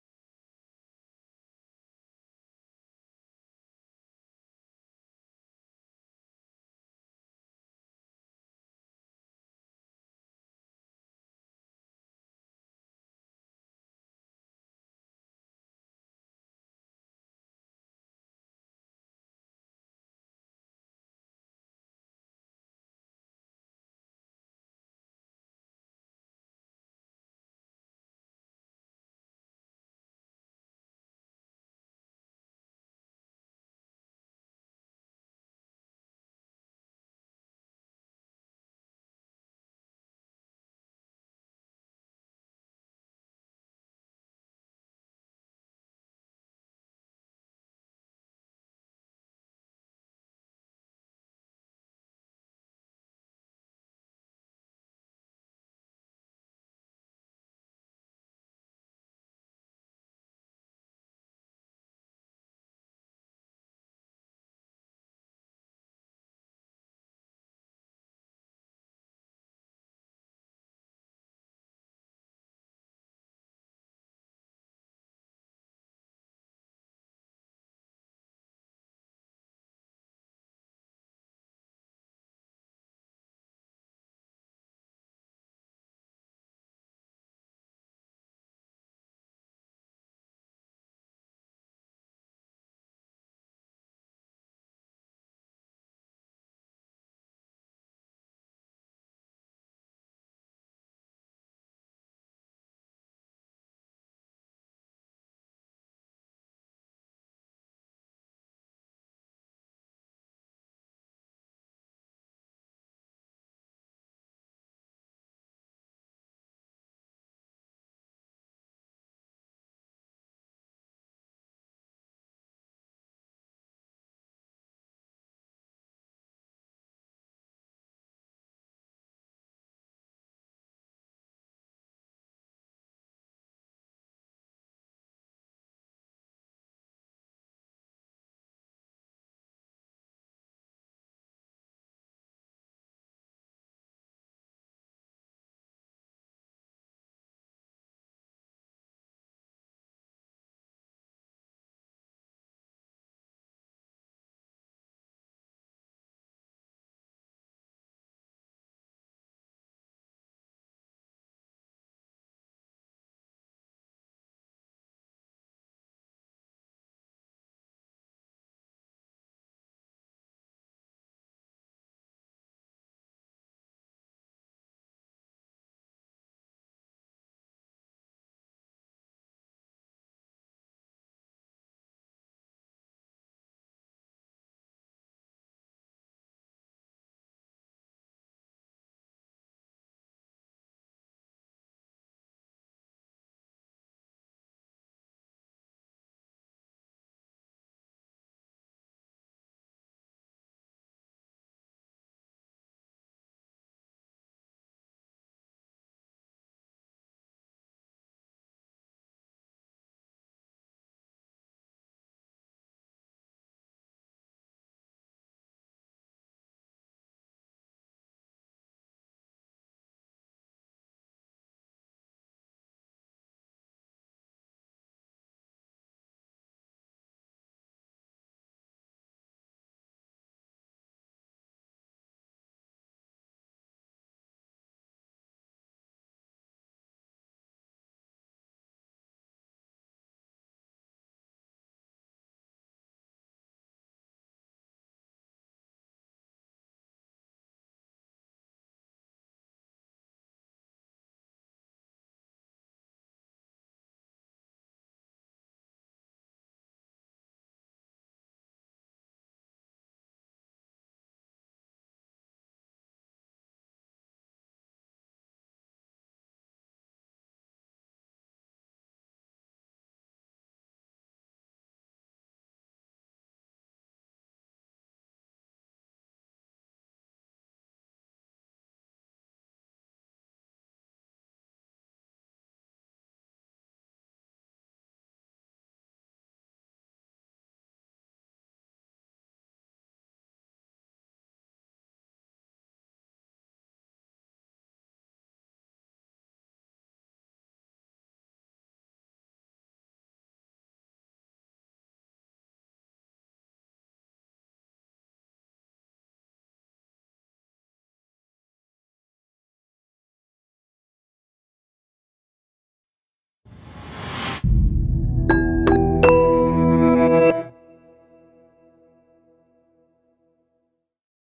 BLUM - SERVO DRIVE FLEX montáž
Potřebujete otevírat bez úchytek lednici, mrazák nebo myčku ?
Využijete SERVO-DRIVE FLEX. Film ukazuje montáž a nastavení.